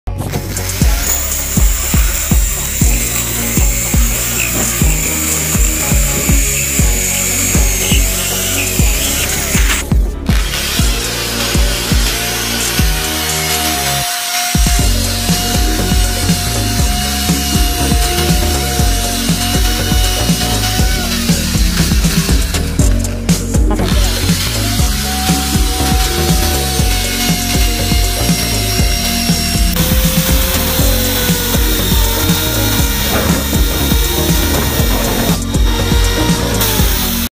Diamond brazing bit drill bit
Diamond brazing bit drill bit resistant marble tile material Ceramic tile Used in granite, marble, rock, stone, shell, etc. Please